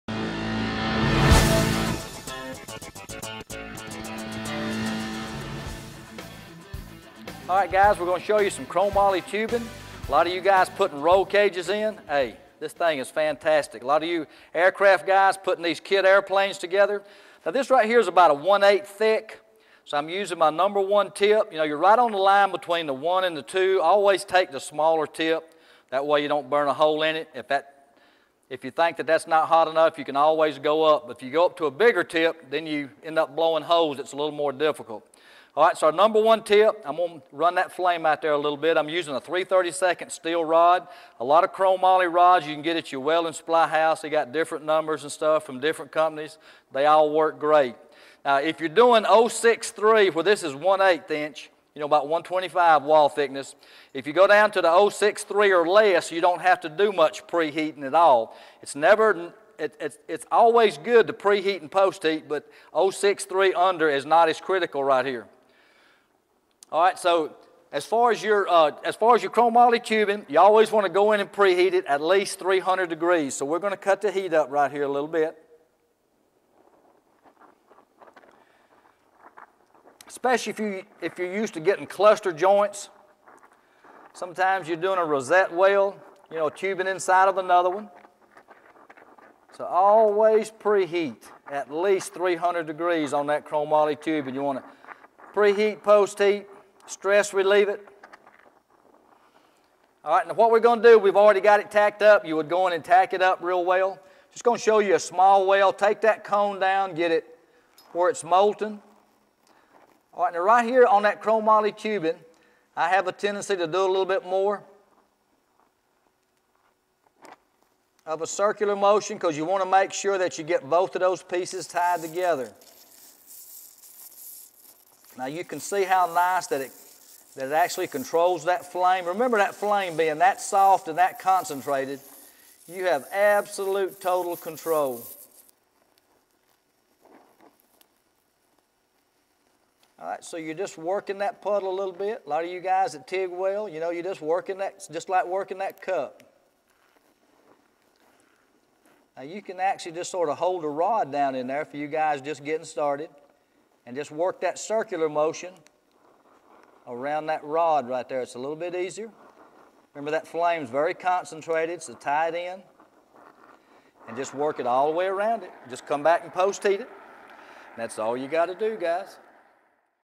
HOW TO GAS WELD CHROMOLY TUBING
A demonstration of how to gas weld chromoly tubing with the DHC2000 oxygen and acetylene welding and cutting torch.